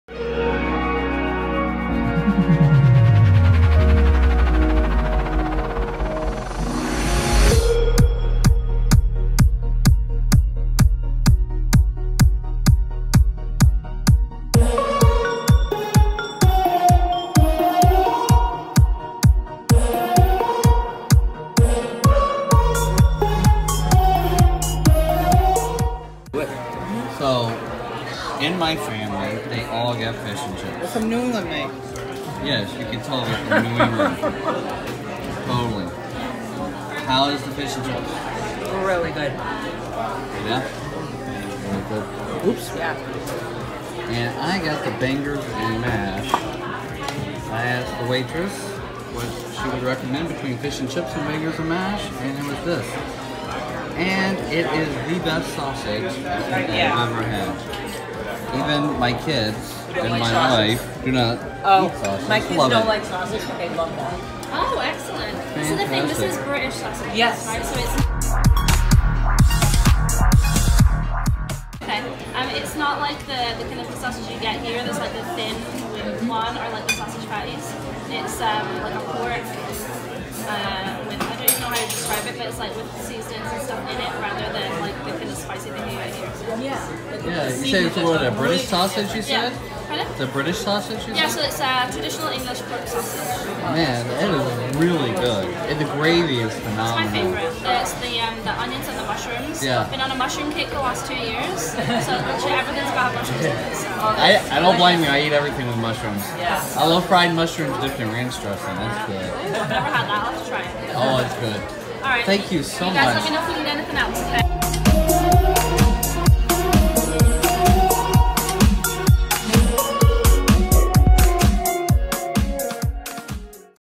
Rose & Crown in Epcot
Lunch in the UK in Epcot. Tried the Bangers and Mash, and it was phenomenal. We ate in the dining room which was a first for us.